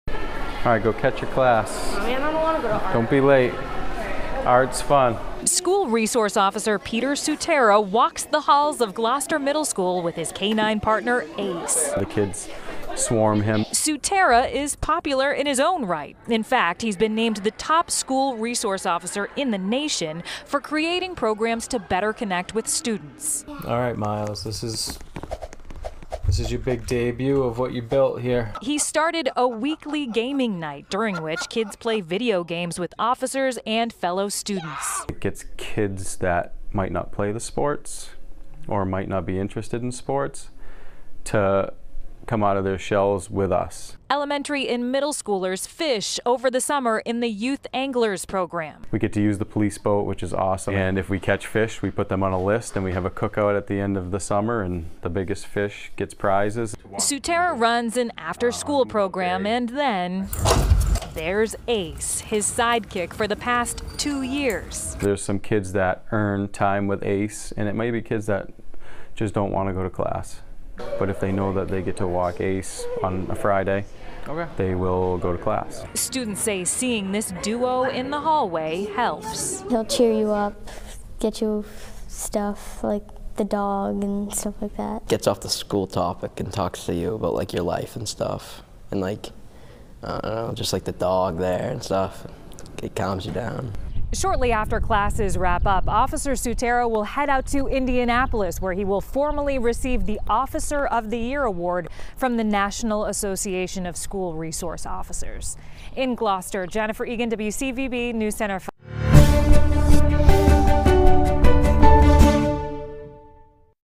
Massachusetts officer named School Resource Officer of the year
Officer Paul Sutera, of the Gloucester Police Department, is being honored as the national school resource officer of the year by the National Association of School Resource Officers.